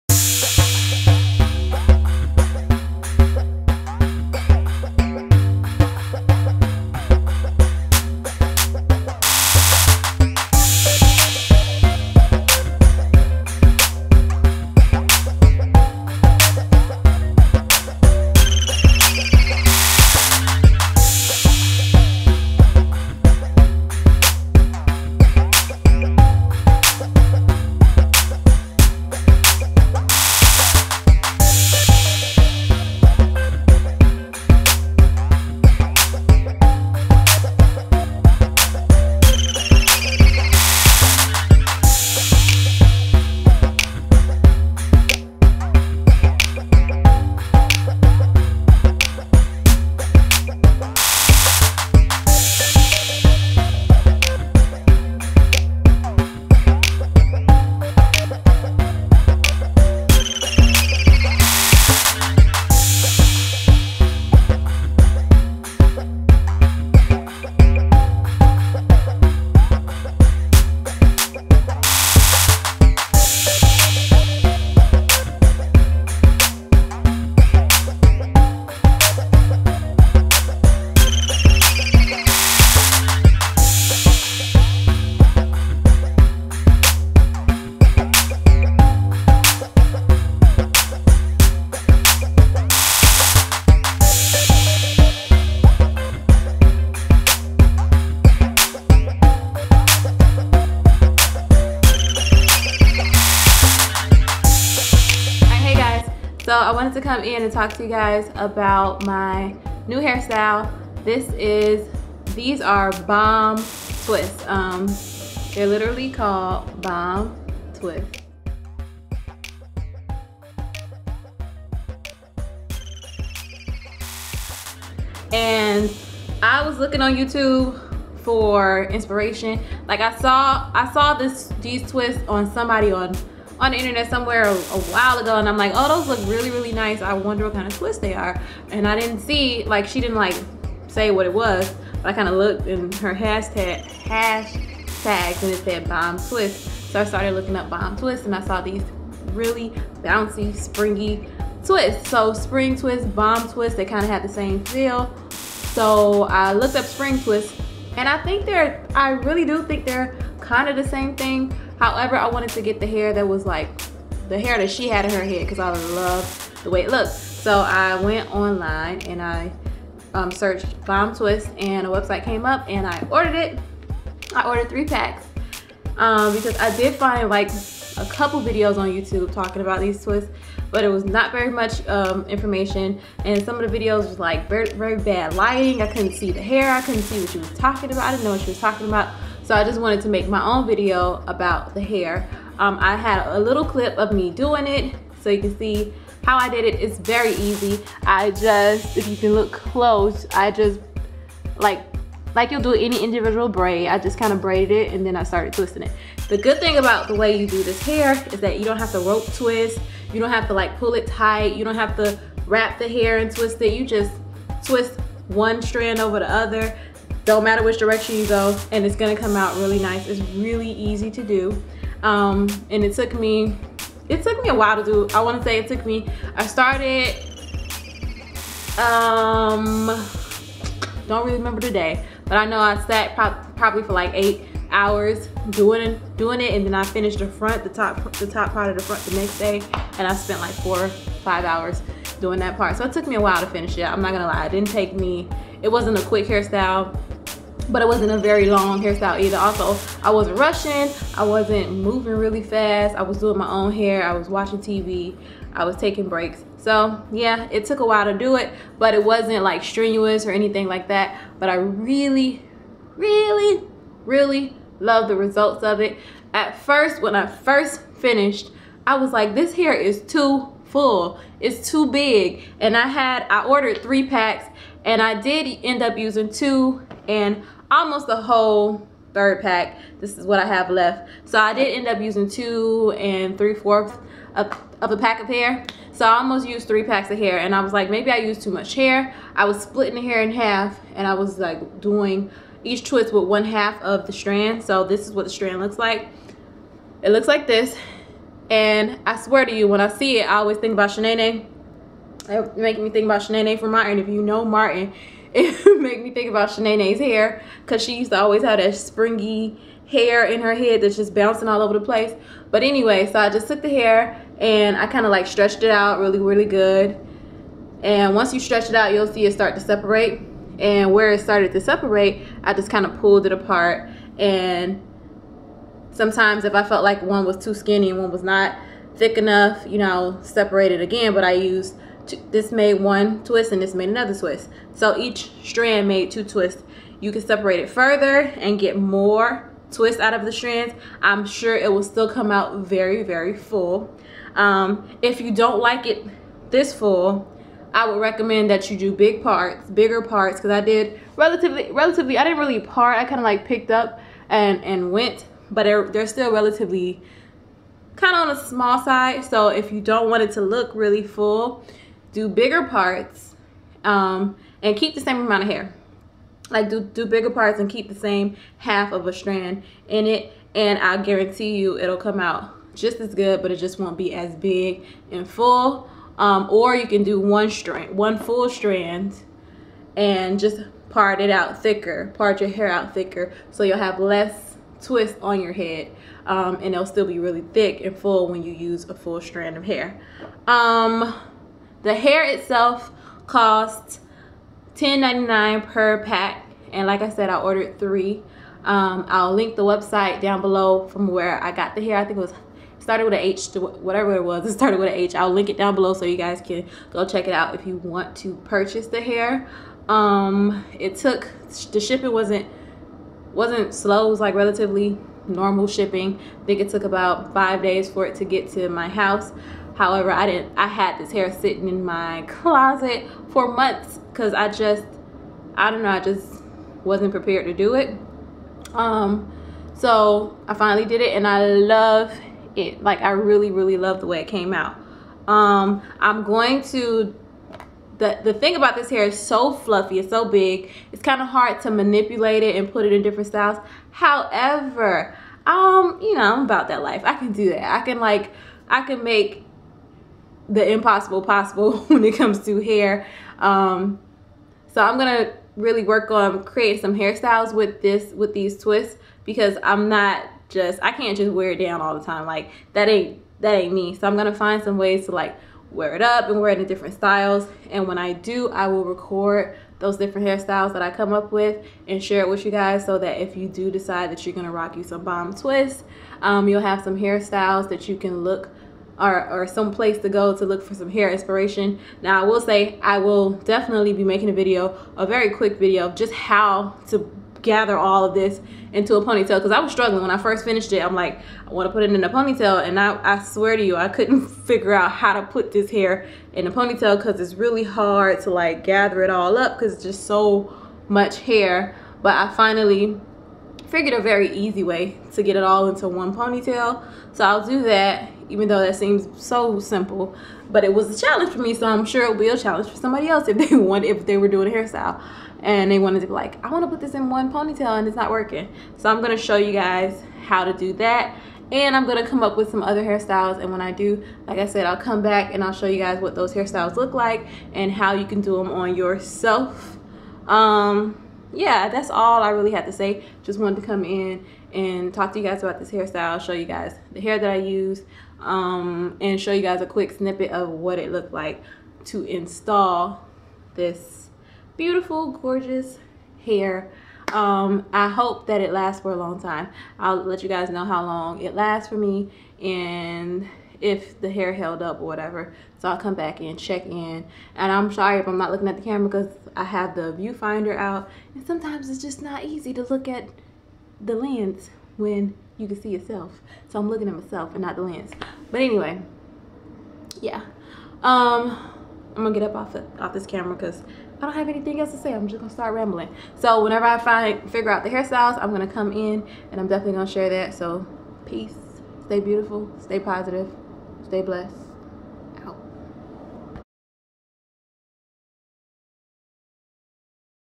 Bomb Twist | Protective Style | Spring Twist
NEW PROTECTIVE STYLE OPEN FOR DETAILS

FAQs

How many packs did I use?
2 and a little more than half of the third

How much is the hair?
10.99 per package

Where can I purchase the hair?
Scroll up to the link

How long did shipping take?
About 5 days (standard shipping)

What color hair did I use?
1B

How long did installation take?
I did my hair over the course of two days as I watched T.V. I did not time myself so I do not know exact times.

How long will you keep the hair in?
4-8 weeks

Does it itch?
No. I did spray diluted apple cider vinegar on the hair the day before i braided it into my hair so that could have contributed to why it is not itchy.

Is it heavy?
No